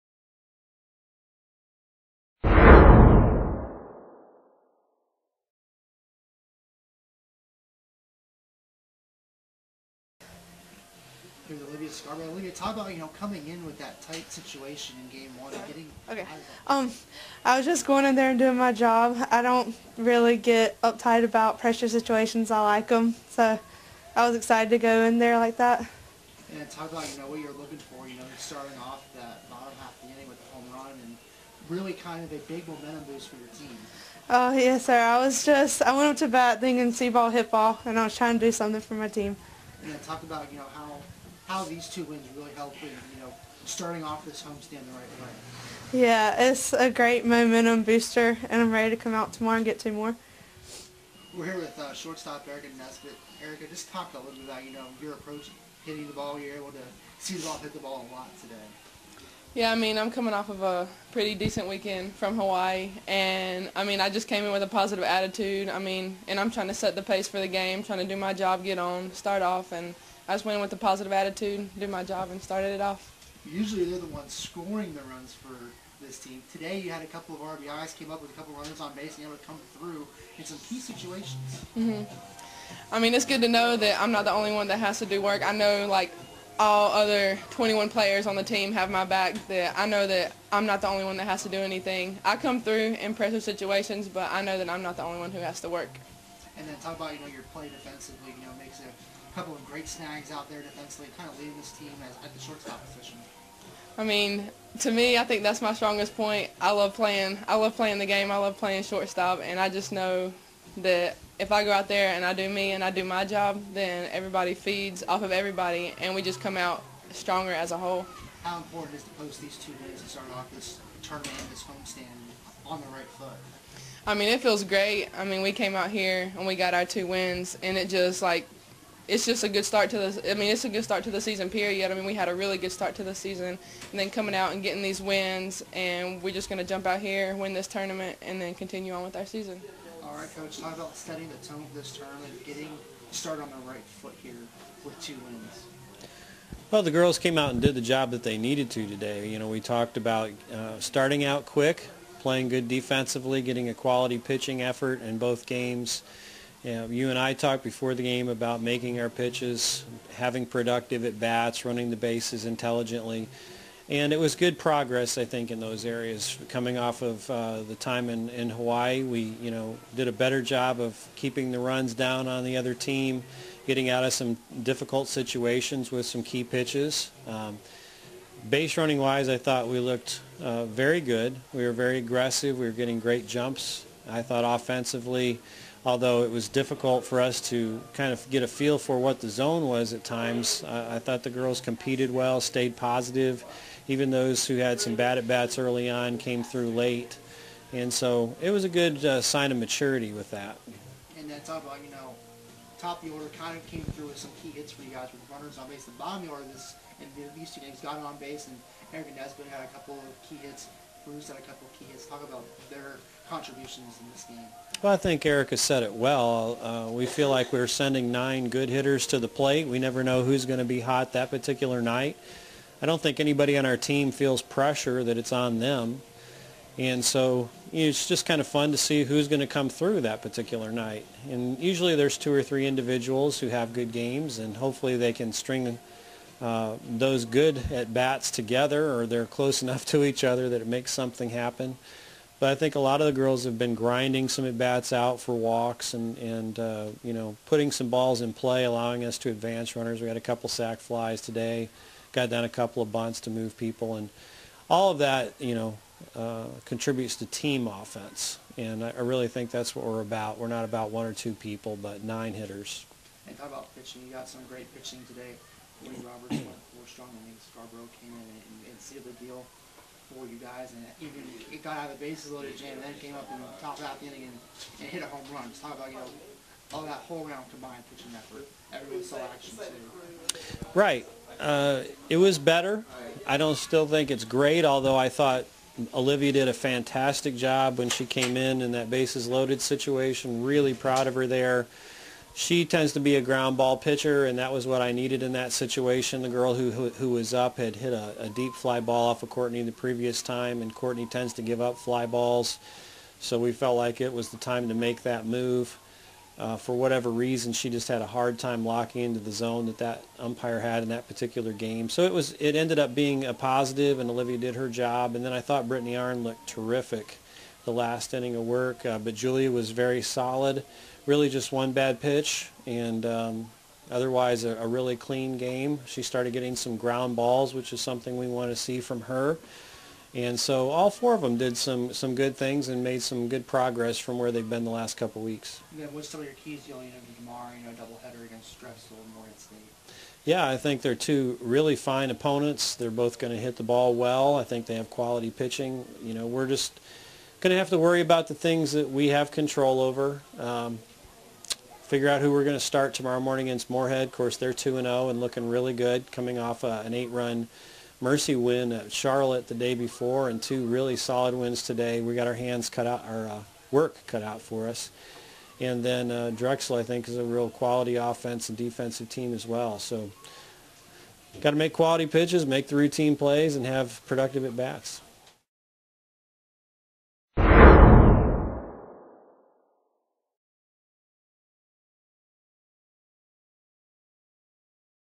Campbell Softball Postgame Camel Stampede Day One - March 8, 2013
Campbell softball head coach Drew Peterson, junior Olivia Scarborough and sophomore Ericka Nesbitt discuss the Camels victories over Northern Kentucky, 6-4, and Norfolk State, 6-2.